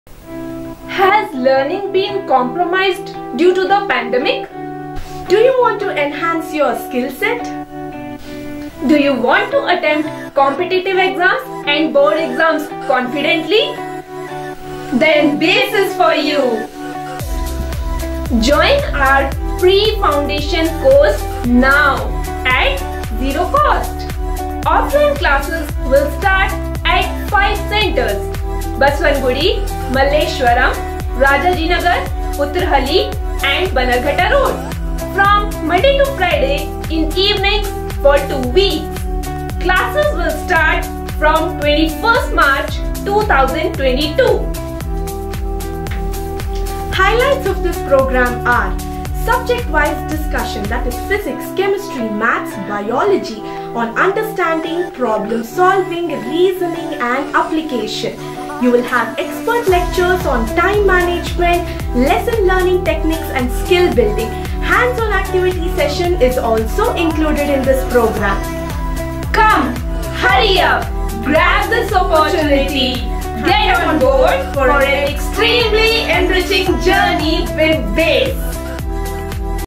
Pre-Foundation Program by BASE
Boost the scientific temperament by adequate exposure to problem solving and applications in PCMB For 7th, 8th and 9th Standard Students.

Join the Pre-Foundation Program by BASE for FREE.

Starting from 21st March 2022, Offline classes will be held at 5 centers (Basavanagudi, Malleshwaram, Rajajinagara, Uttharahalli, Banneraghatta Road).

Program Features:
- From Monday to Friday, evening classes for two weeks (2 hrs per day, 20 hrs program)
- Subject wise (PCMB) discussion on understanding, reasoning, problem solving and application
- Expert lecture on time management, skill building and learning techniques
- Hands on activity session included